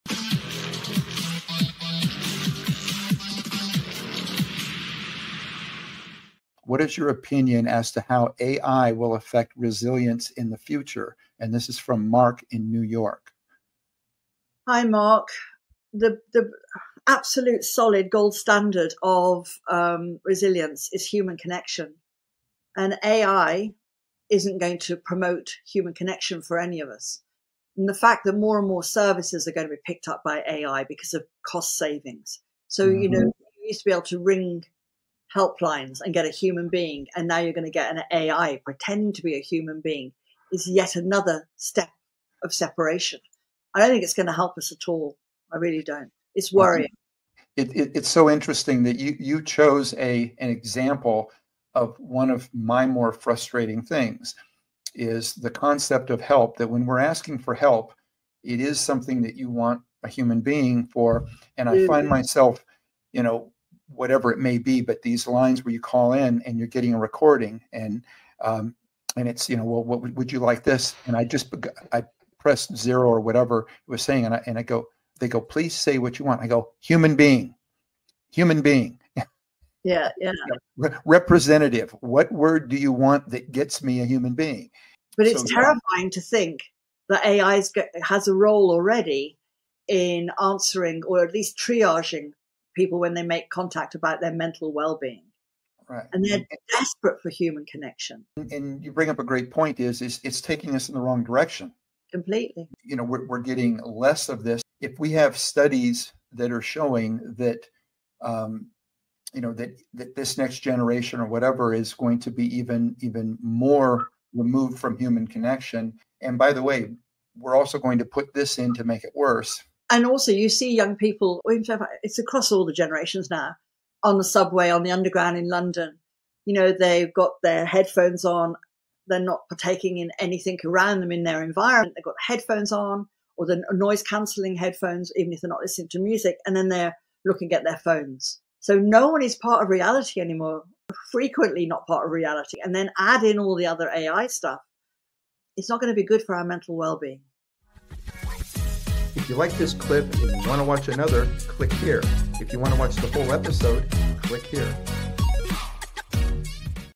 AI Threatens Mental Health?? 🤯🤯🤯
This highlight explores the significant concern of AI's impact on human resilience and connection.

Sally Baker delves into the growing role of AI in daily life and its implications for mental health and social behaviors. We discuss the potential erosion of human connections due to increased AI integration in support systems, examining whether this technological shift is steering us away from essential human interactions and how it might affect our mental well-being and societal fabric.